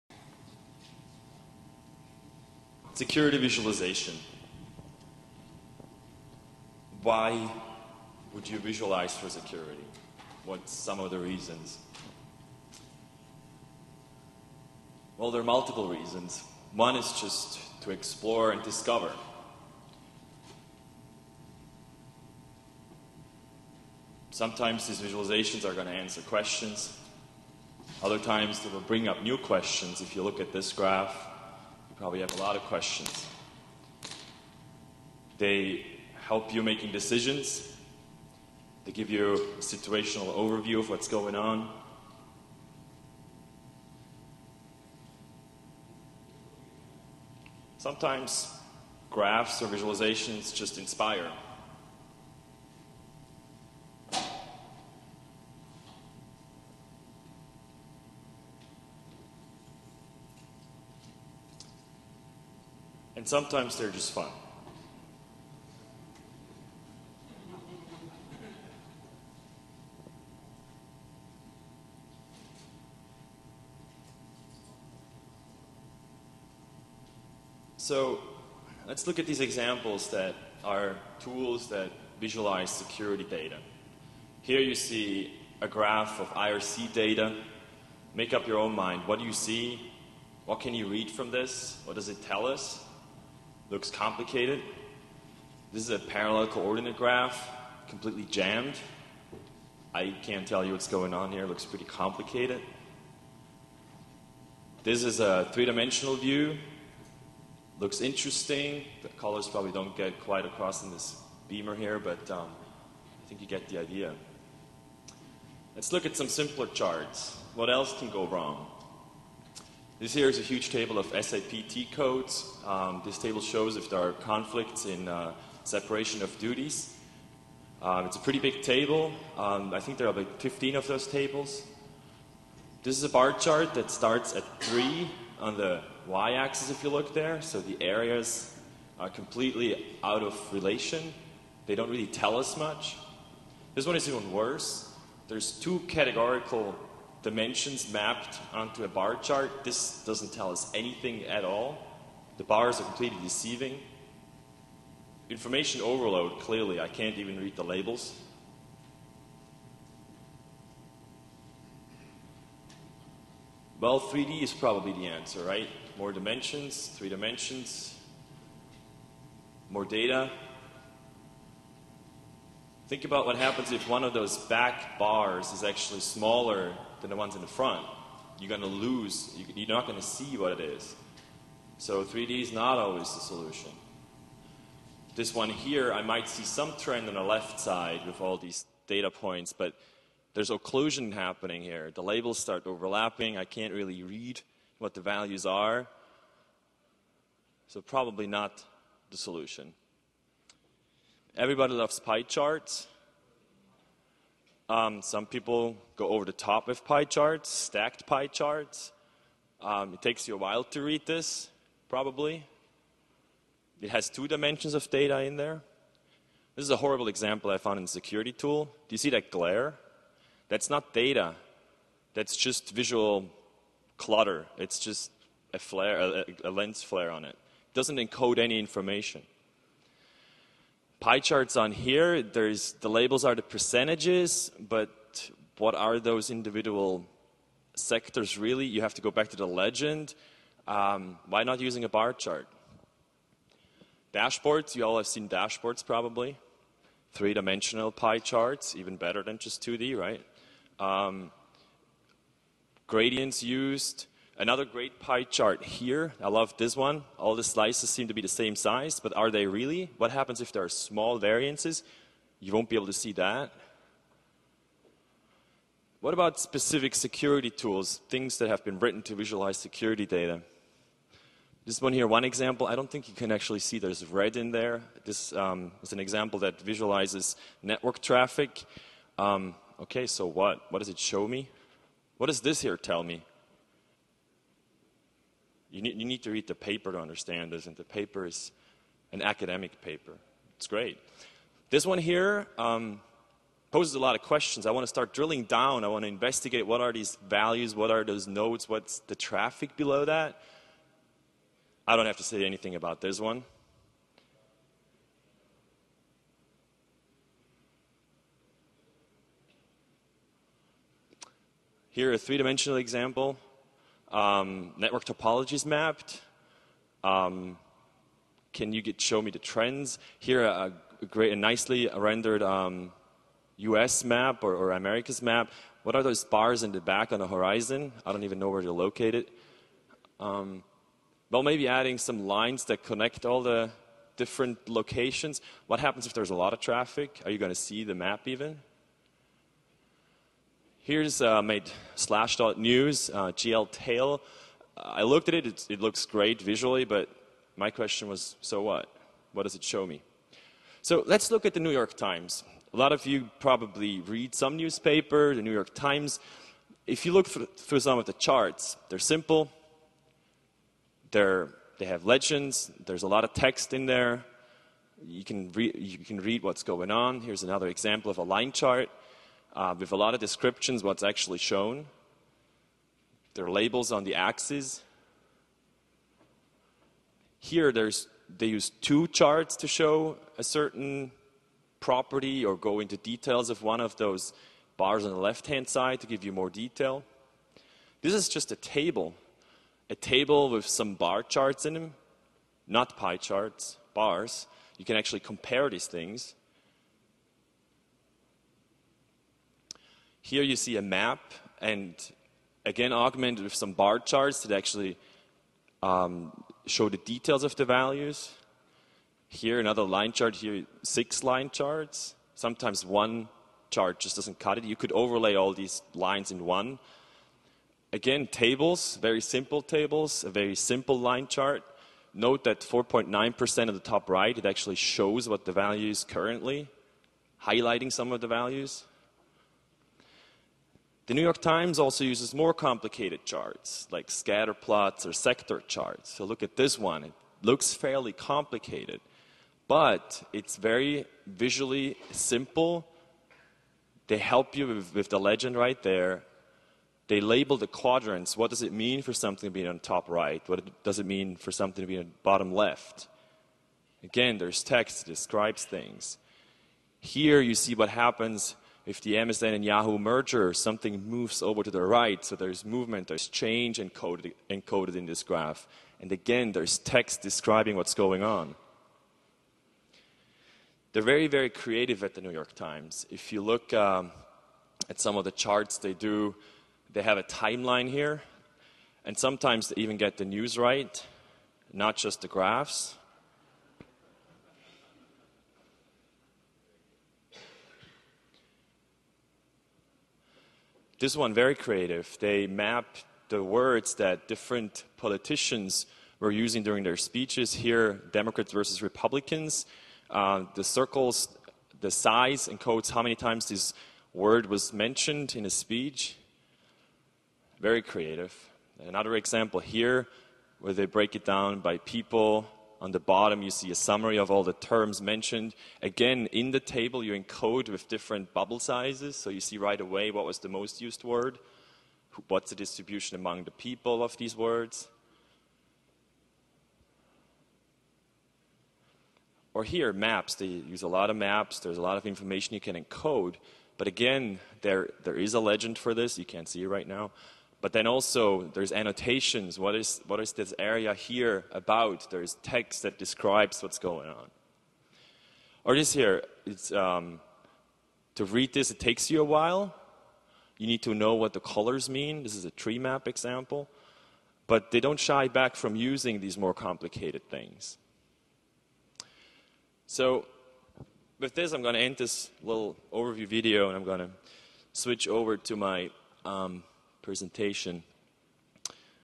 Security Visualization - Learning From The New York Times
All the data that's fit to visualize. What can security visualization learn from the New York Times?
The video shows some bad examples of security visualizations and what the NYT does to visualize data. How can secviz learn from that?